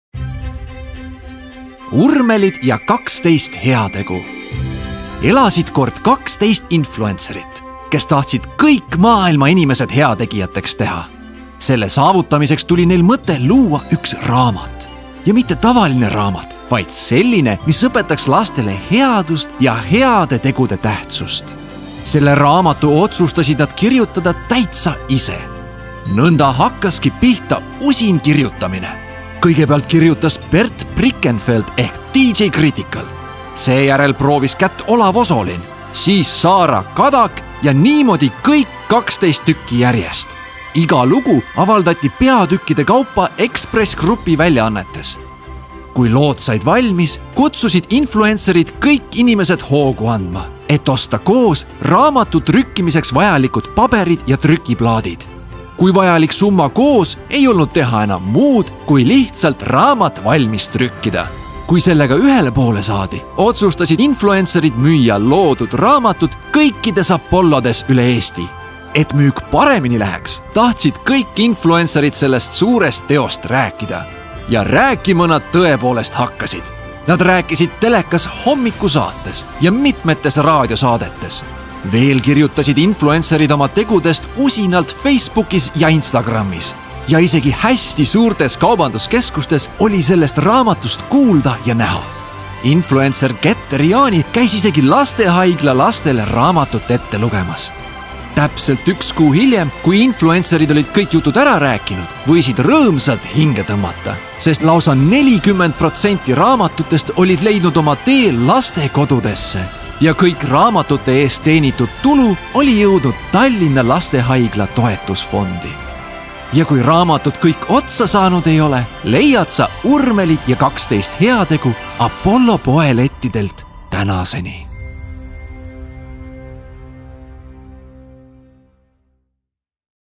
Urmelid ja kaksteist heategu
Lõime koos 12 armastatud inimesega Tallinna Lastehaigla maskottidest uue lasteraamatu. Heategevuslik raamat räägib urmelitest, toetab Lastehaiglat müügituluga ja õpetab lapsi head tegema.

Raamatu on kirjutanud Evelin Ilves, Janek Mäggi, Olav Osolin, Bert Prikenfeld, Jaagup Kreem, Saara Kadak, Kristel Aaslaid, Katrin Viirpalu, Yoko Alender, Getter Jaani, Mikk Jürjens ja Raivo E. Tamm.